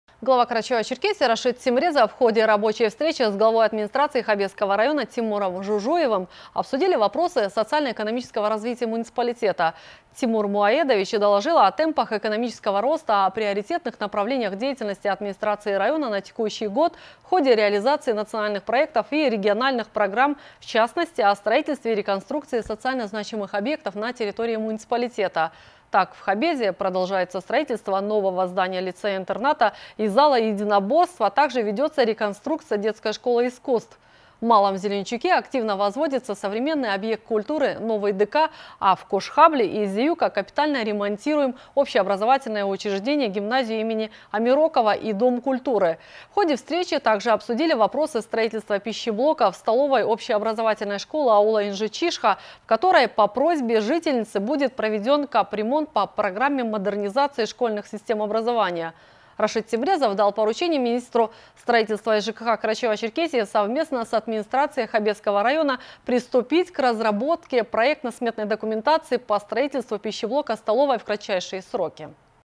Рашид Темрезов обсудил в Тимуром Жужуевым перспективы развития Хабезского района
Руководитель района рассказал главе Карачаево-Черкесии о ряде направлений деятельности муниципальных властей по обеспечению экономического роста, а также о реализации национальных проектов и региональных программ. В частности – речь шла о продолжении строительства в Хабезе нового здания лицея-интерната и зала единоборств, а также о реконструкции детской школы искусств. Тимур Жужуев сообщил Рашиду Темрезову и о возведении в Малом Зеленчуке нового ДК, а также о капитальном ремонте в Кош-Хабле и Зеюко гимназии им. Амирокова и дома культуры.
Кроме того, в ходе встречи были обсуждены вопросы предстоящего капитального ремонта школы в ауле Инжичишхо, а также строительство пищеблока в данном образовательном учреждении.